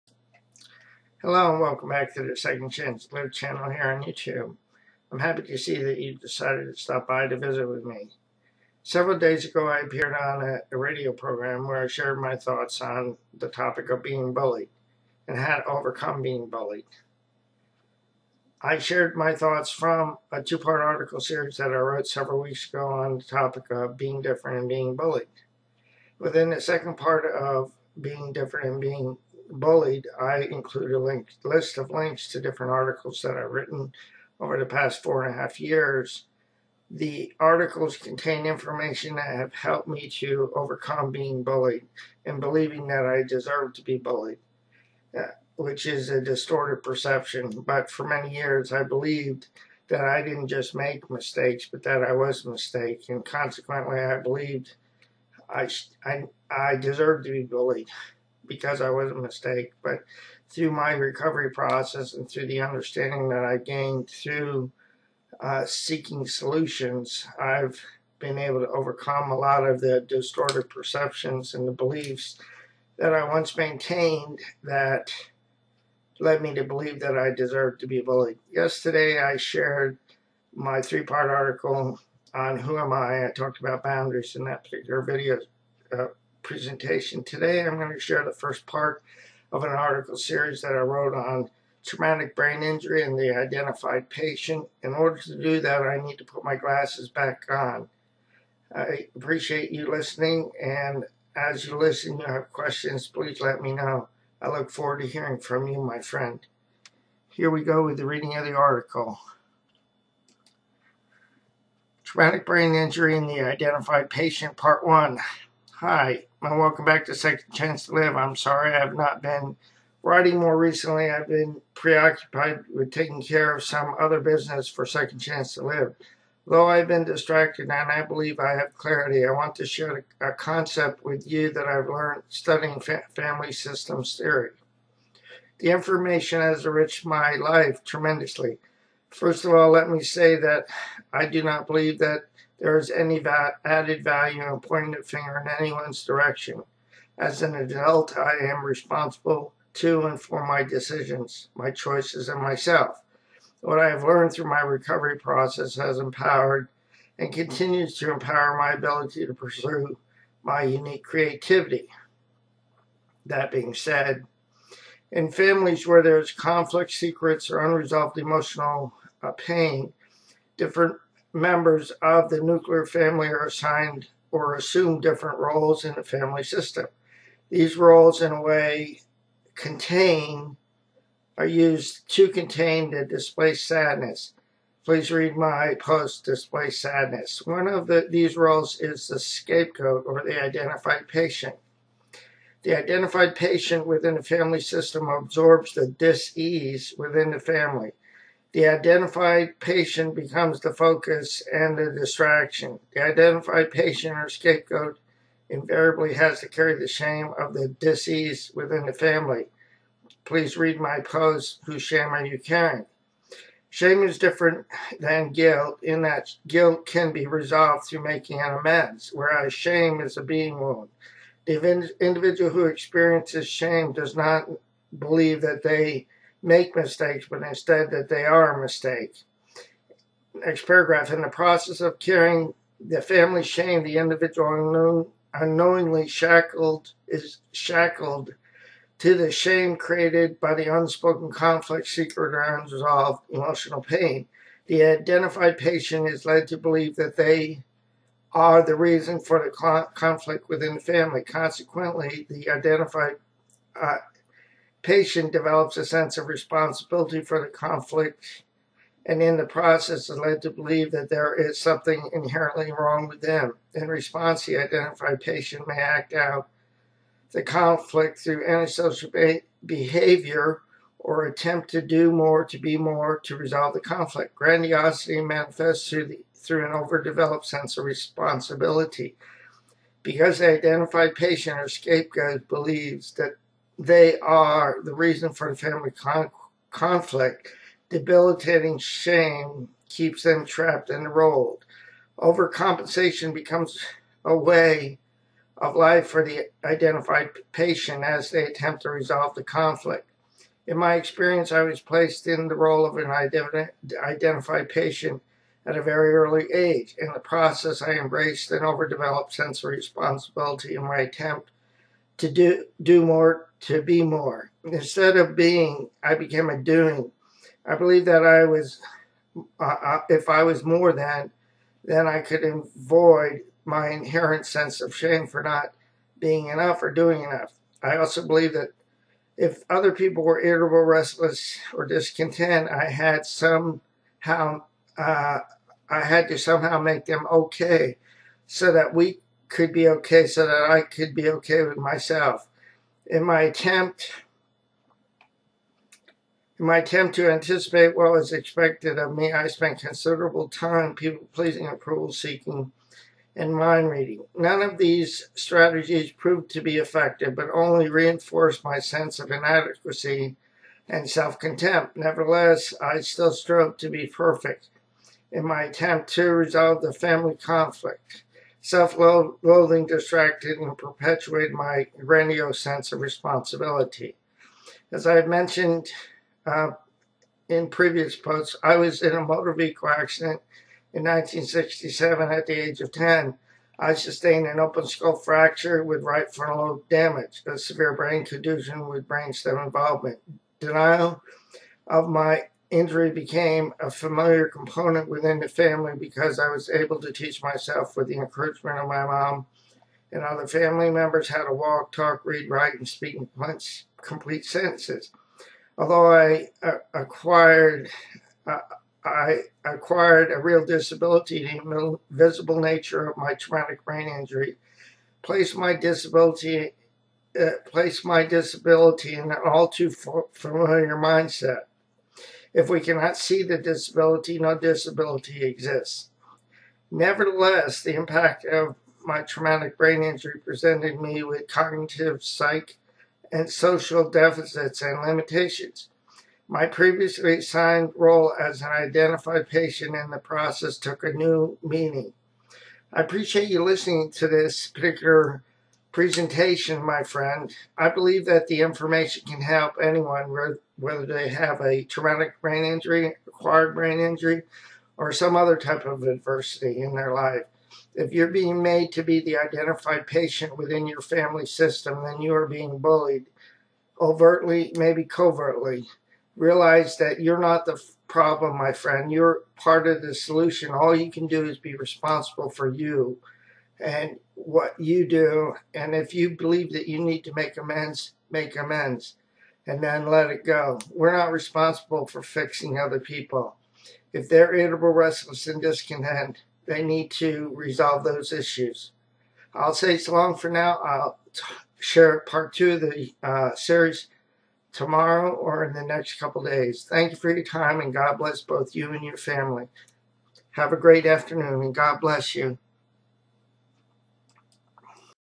Traumatic Brain Injury and The Identified Partient Part 1 Episode 23
In this presentation I share what I learned that set me up to believe that I deserved to be bullied. In Part 2 of this presentation, I will share how I  overcame the beliefs that set me up to be bullied.